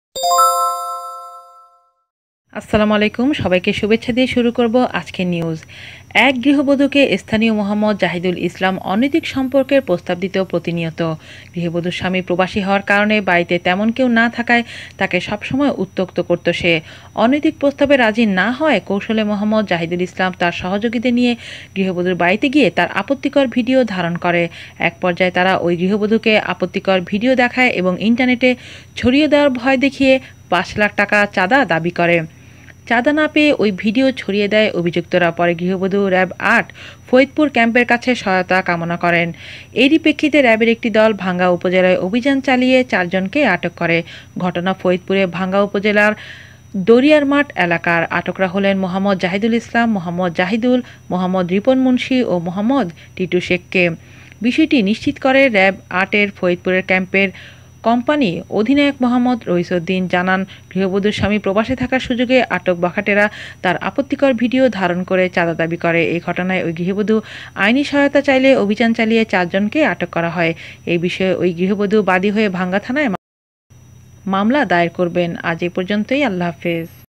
প্রবাসীর স্ত্রীর আপত্তিকর ভিডিও ইন্টারনেটে ছড়িয়ে দেয়ার ভয় দেখিয়ে ৫ লাখ টাকা নিলো!!
অনৈতিক প্রস্তাবে রাজি না হওয়ায় কৌশলে মো. জাহিদুল ইসলাম তার সহযোগীদের নিয়ে গৃহবধূর বাড়িতে গিয়ে তার আপত্তিকর ভিডিও ধারণ করে। এক পর্যায়ে তারা ওই গৃহবধূকে আপত্তিকর ভিডিও দেখায় এবং ইন্টারনেটে ছড়িয়ে দেয়ার ভয় দেখিয়ে ৫ লাখ টাকা চাঁদা দাবি করে।

চাঁদা না পেয়ে ওই ভিডিও ছড়িয়ে দেয় অভিযুক্তরা। পরে গৃহবধু র‌্যাব-৮ ফরিদপুর ক্যাম্পের কাছে সহায়তা কামনা করেন। এরই প্রেক্ষিতে র‌্যাবের একটি দল ভাঙ্গা উপজেলায় অভিযান চালিয়ে চারজনকে আটক করে।